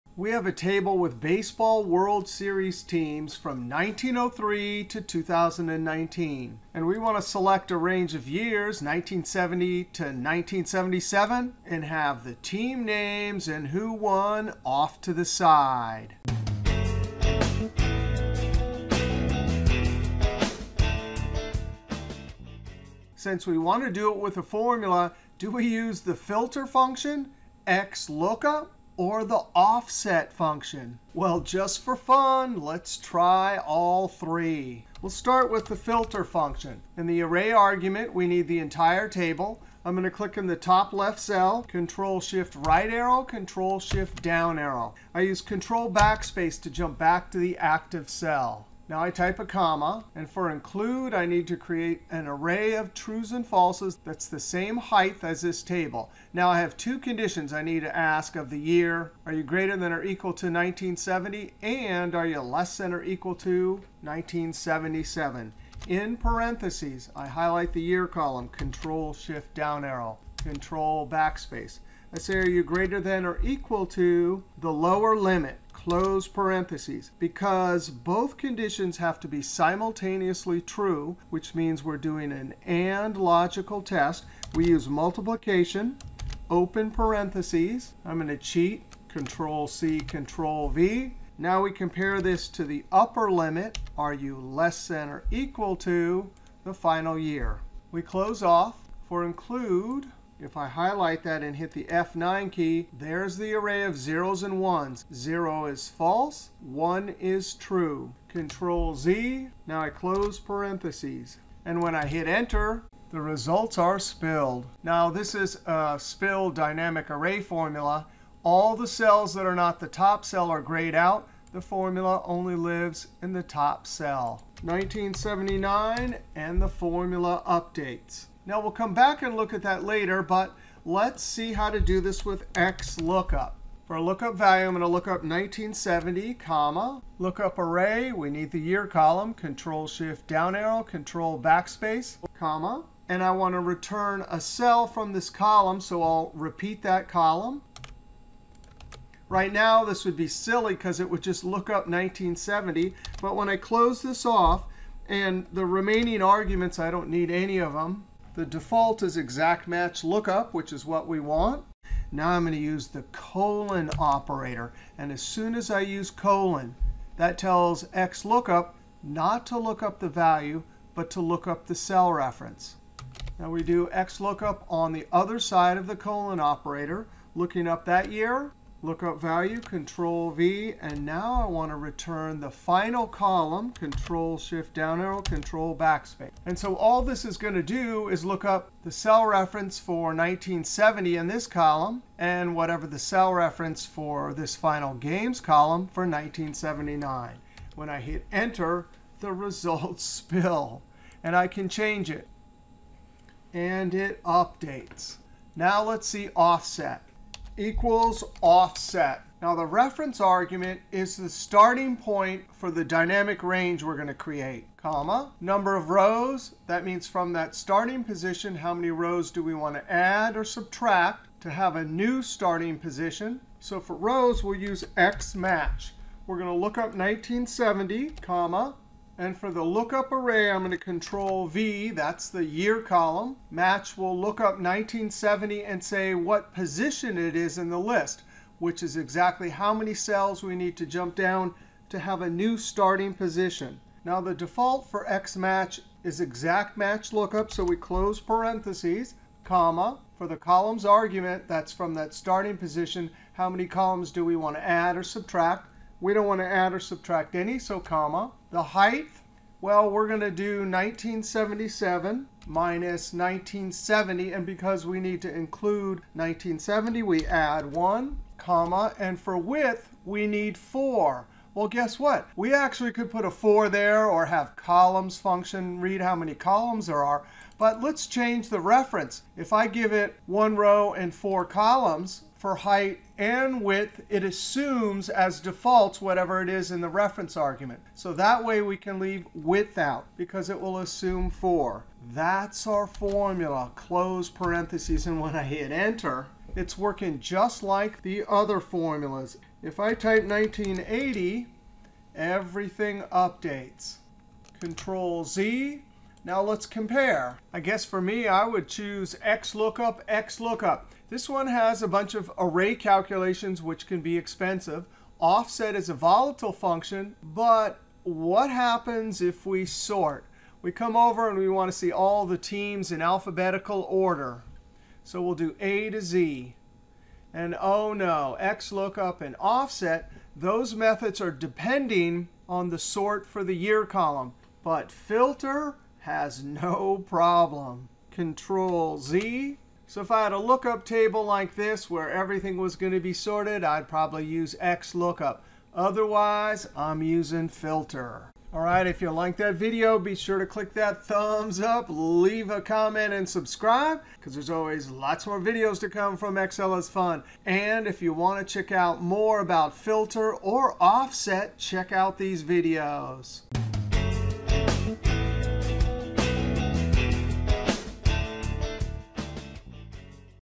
OFFSET, XLOOKUP or FILTER function for Extracting Baseball Data? Excel Magic Trick 1697.
Learn how to extract baseball World Series Baseball Records based on a start and end year using one of three functions: FILTER, XLOOKUP or OFFSET? We see XMATCH function also.
Topics:
1. (00:00) Introduction.
2. (00:22) Which function?
3. (00:35) FILTER function.
4. (02:10) XLOOKUP function.
5. (03:27) OFFSET function.
6. (05:06) Compare all methods.
7. (05:48) Summary, Closing and Video Links